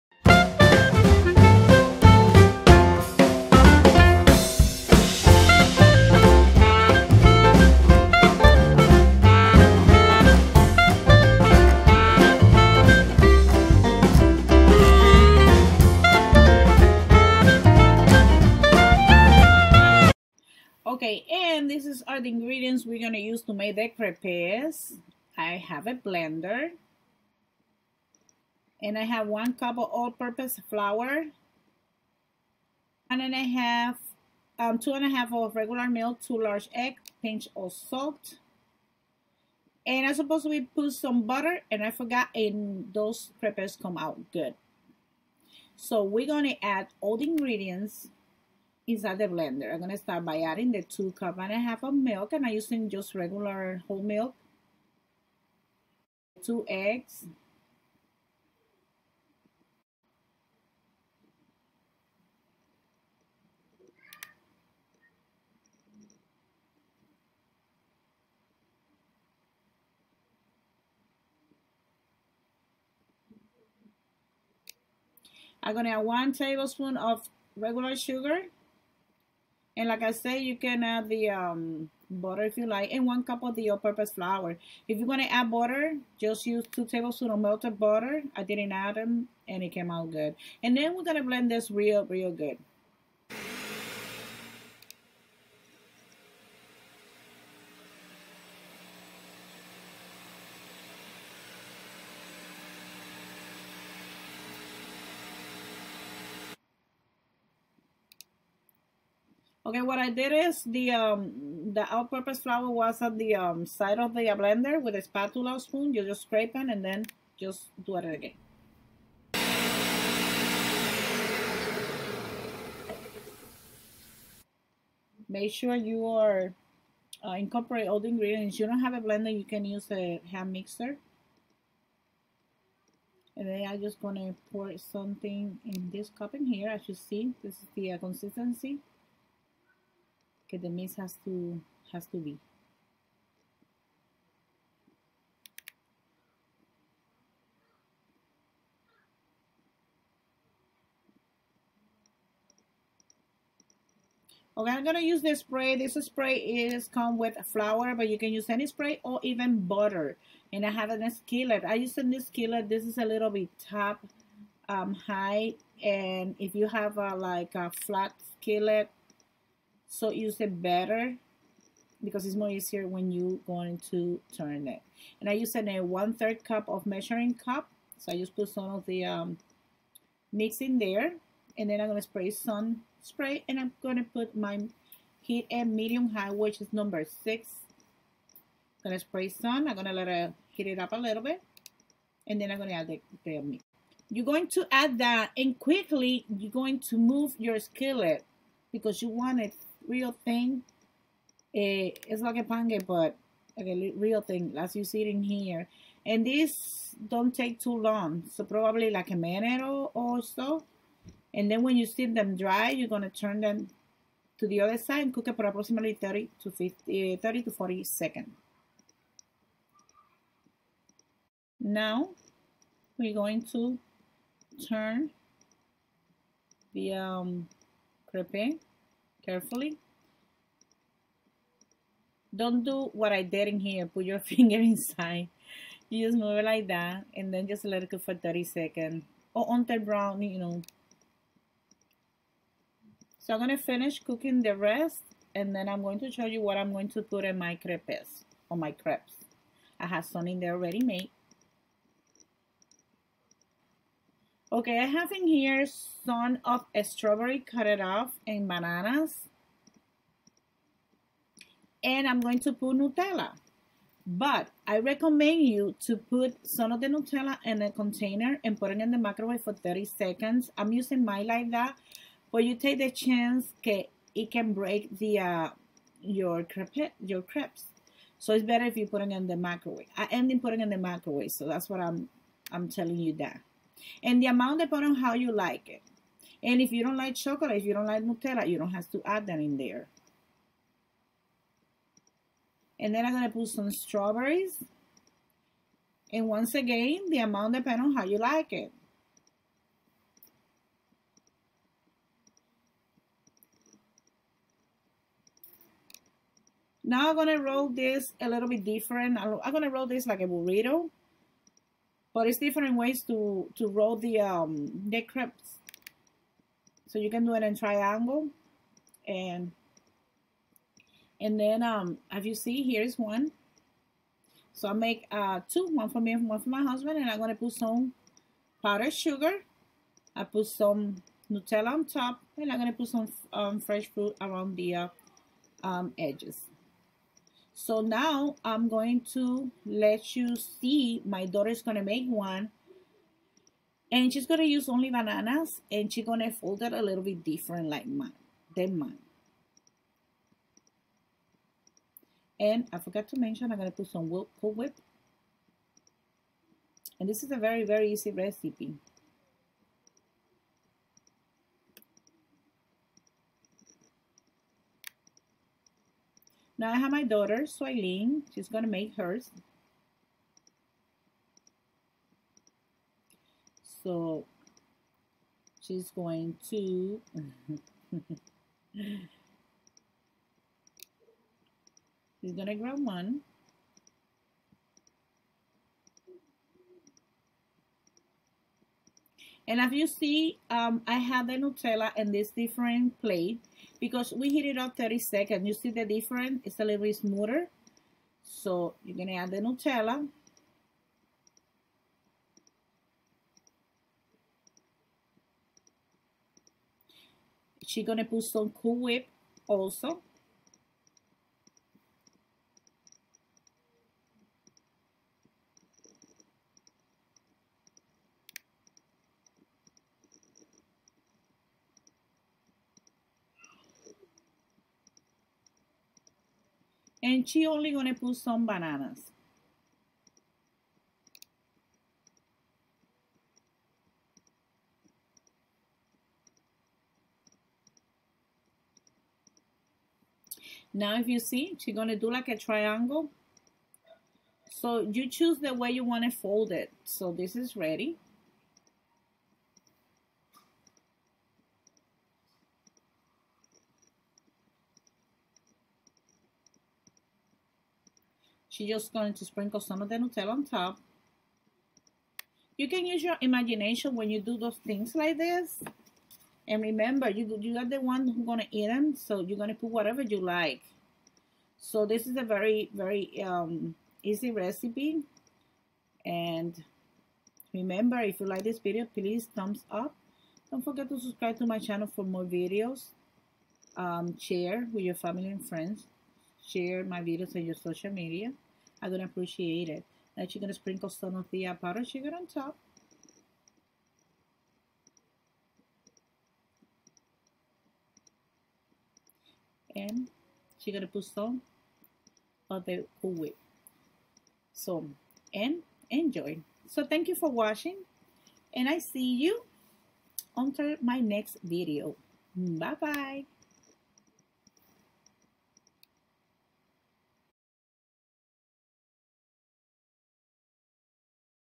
How to Make Crepes with Nutella ,fresh fruits and Cool Whip
Ingredients
Blender or hand Mixer
1 cup of all purpose flour
2 1/2 whole milk
2 large eggs
pinch of salt
1 tablespoon sugar
Cooking Spray
Skillet
any failing you like like fresh fruit I Used Strawberries and bananas
Confectioner sugar (Optional )
Nutella
Cool Whip